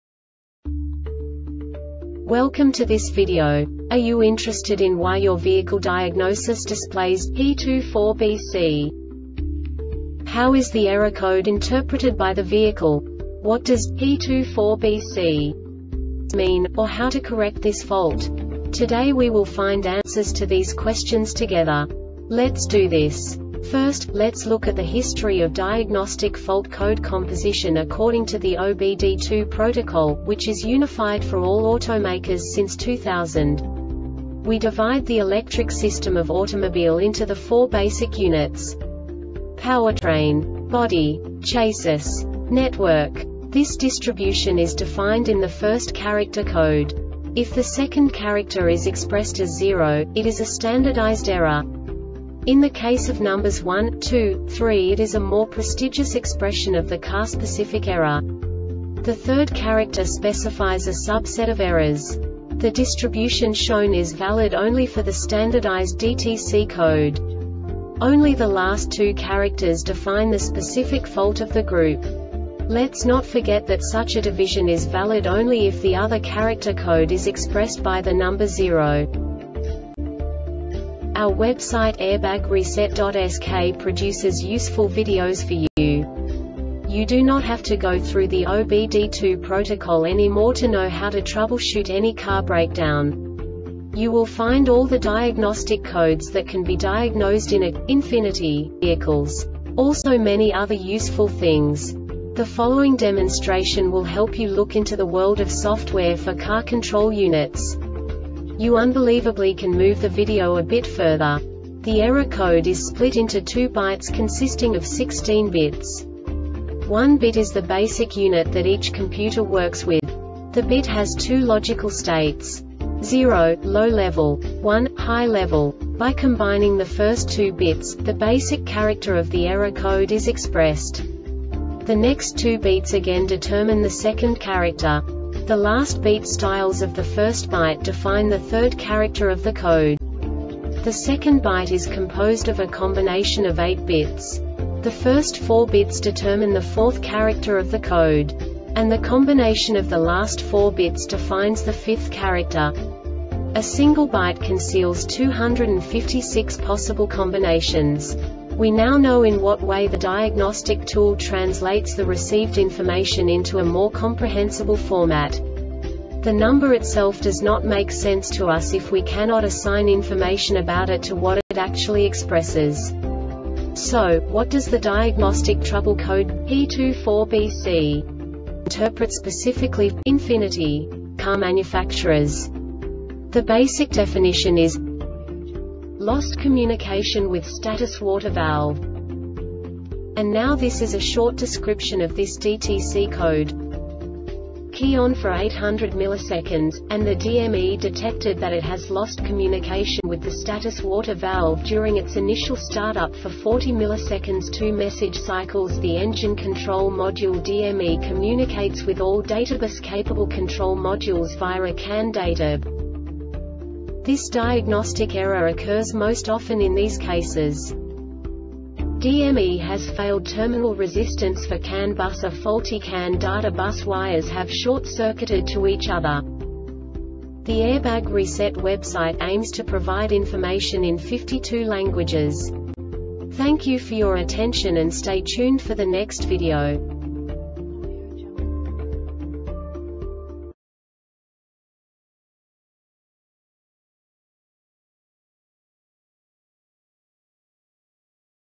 DTC Ford P24BC Short Explanation
Contents:
0:21 Basic DTC analysis according to OBD2 protocol standard.
1:48 Insight into programming
2:58 A diagnostically understandable description of the Diagnostic Trouble Code
3:05 Basic error definition

Make:
Ford
Code:
P24BC
Definition:
Lost Communication With Status Water Valve
Description:
Key on for 800ms,and the DME detected that it has lost communication with the status water valve during its initial startup for 40ms (2 message cycles).The Engine Control Module (DME) communicates with all databus-capable control modules via a CAN datab
Cause: